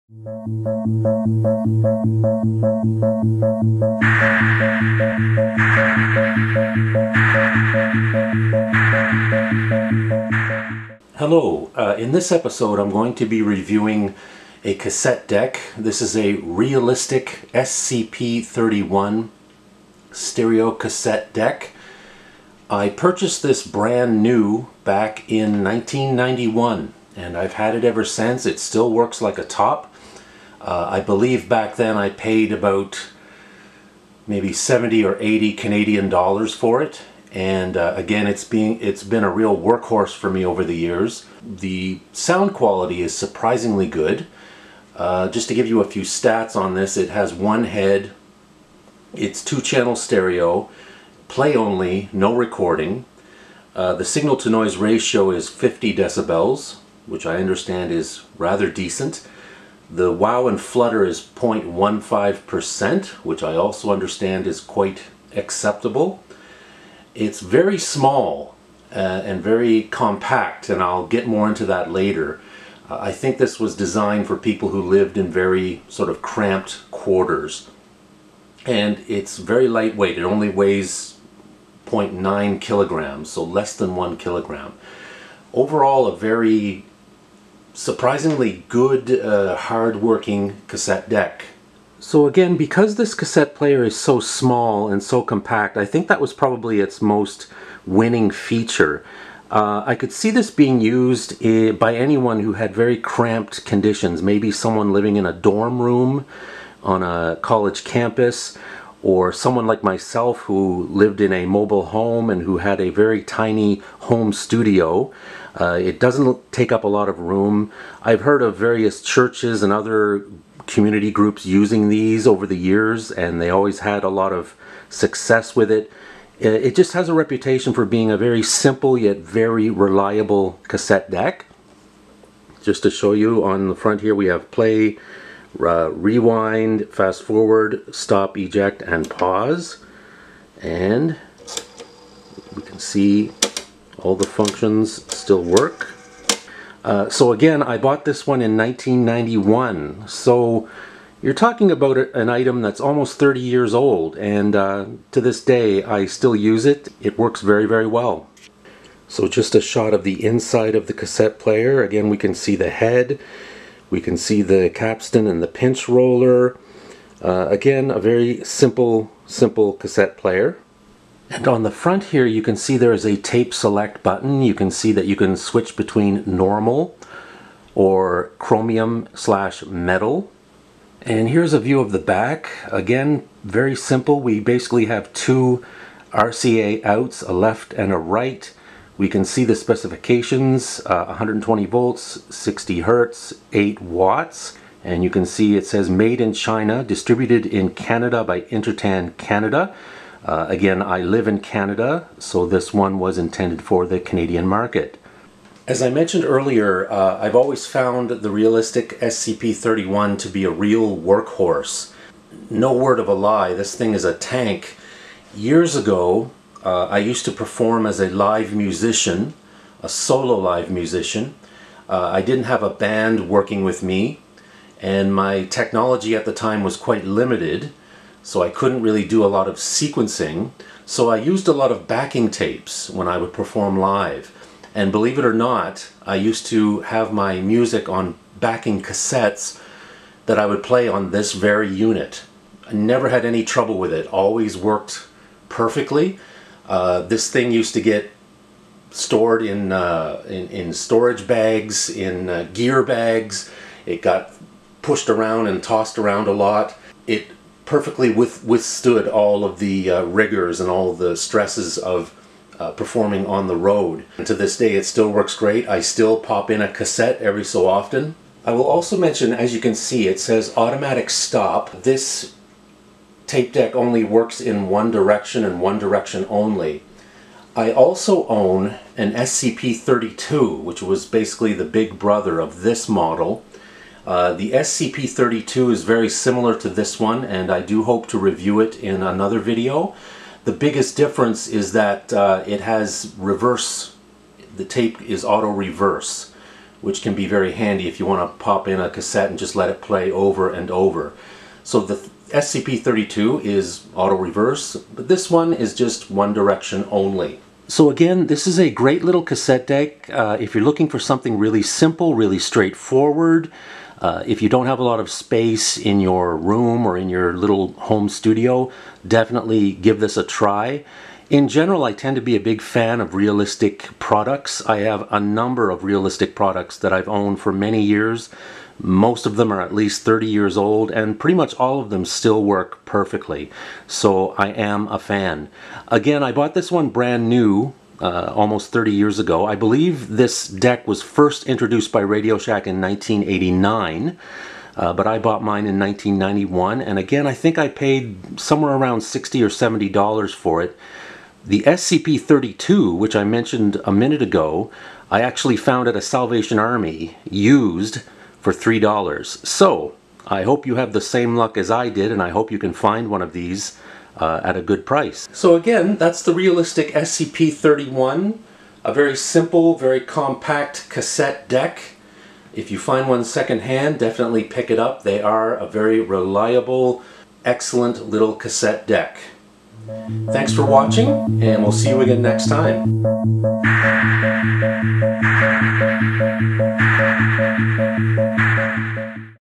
Tech Review: Realistic SCP 31 Cassette Player | Vintage Home Audio
My review of this small but reliable tape deck from 1989.  If you are a cassette collector and you enjoy vintage home audio, this tape deck is for you.  In this tech review, I take a look at this retro Radio Shack cassette player.

This would certainly not be considered an audiophile cassette deck, however in terms of consumer grade players it works just fine.  This retro tape deck is fun to use and easy to operate.  If you remember the Realistic line of gear sold by Radio Shack, you know that it was often more reliable than most people gave it credit.

People who are still into cassettes and cassette tape collectors should seek this one out.

cassette player
Radio Shack cassette player/recorder
vintage cassette deck reviews
80s cassette player